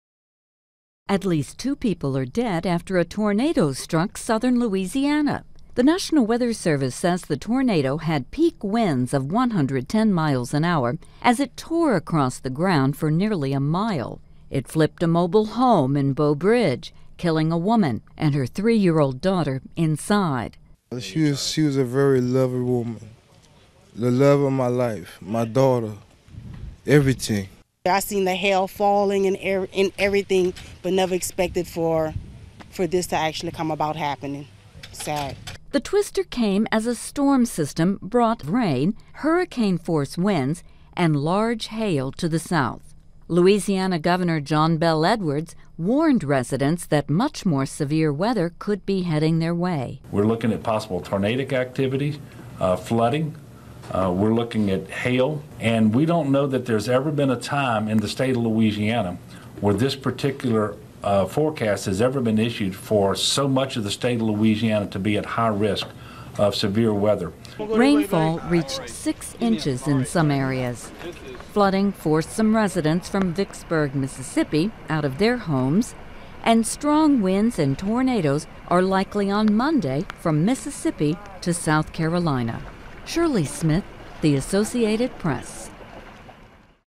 Deadly Tornado in Louisiana as Storms Sock South
A tornado is blamed for at least two deaths in southern Louisiana. It overturned a trailer home, killing a mother and daughter. Dangerous weather is expected from Mississippi to South Carolina on Monday. (April 3)